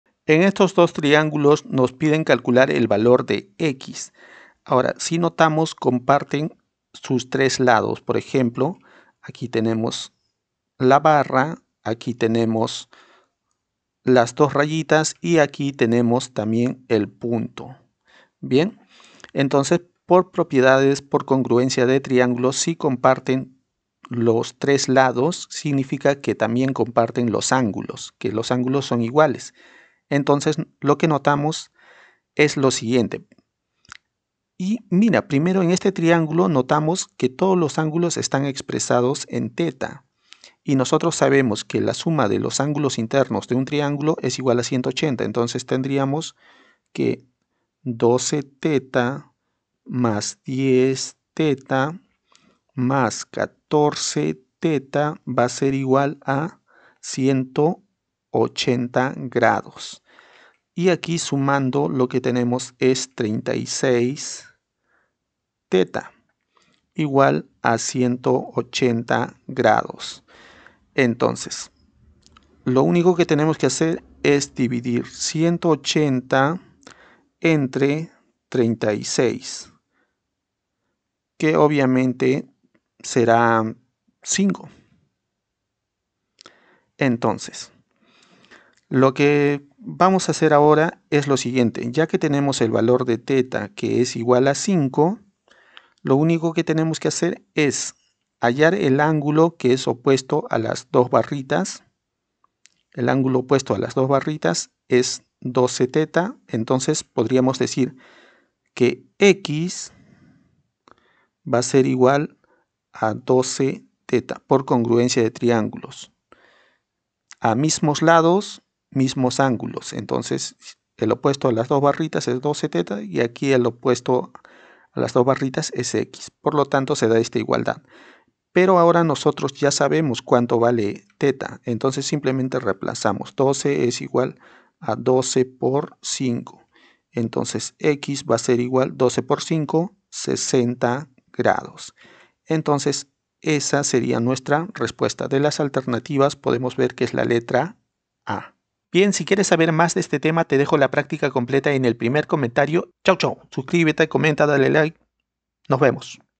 Hallar “x” - Congruencia y Semejanza de triangulos - ejercicios
Hallar “x” - Congruencia y Semejanza de triangulos - ejercicios de  geometria paso a paso, ejercicios de triangulos paso a paso, aprende geometria nivel colegio y preuniversitario de manera divertida y super grafica aplicando propiedades y formulas